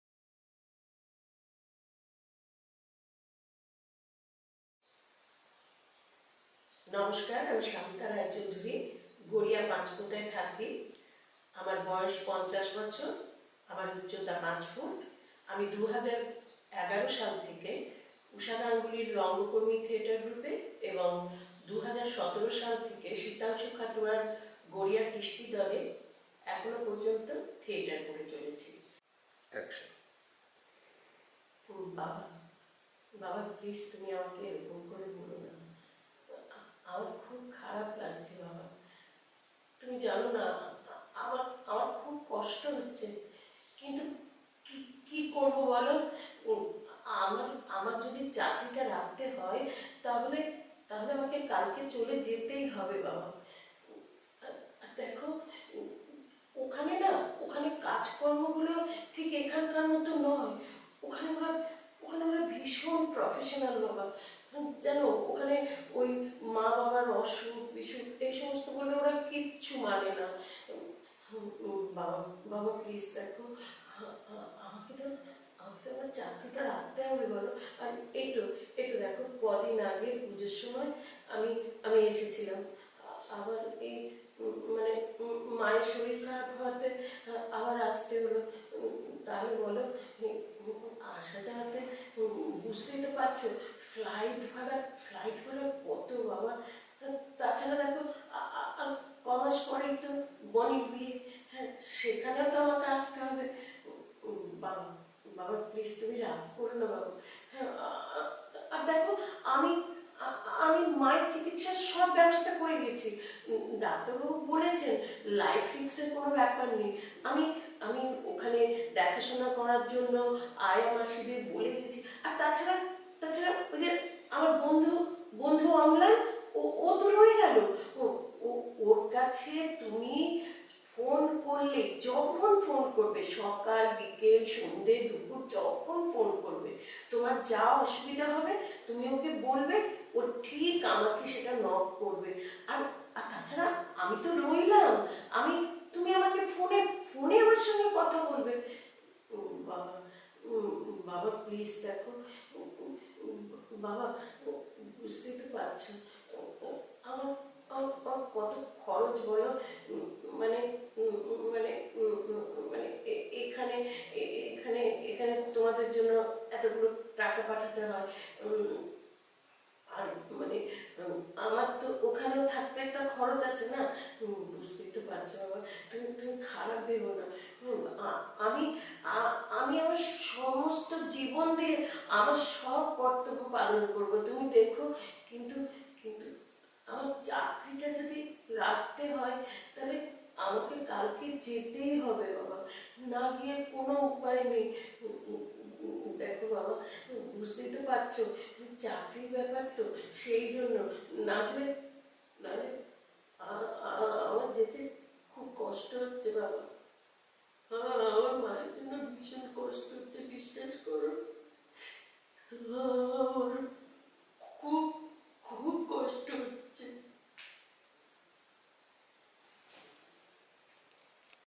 Audition of Santa Roy Chowdury (50, 5') For Bengali Serial | Kolkata | Tollywood Industry.com
Santa Roy Chowdury a talented actress attends the audition of Bengali Serial for renowned channel, which is organised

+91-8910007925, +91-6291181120

Office Address:
'Perfect Solutions'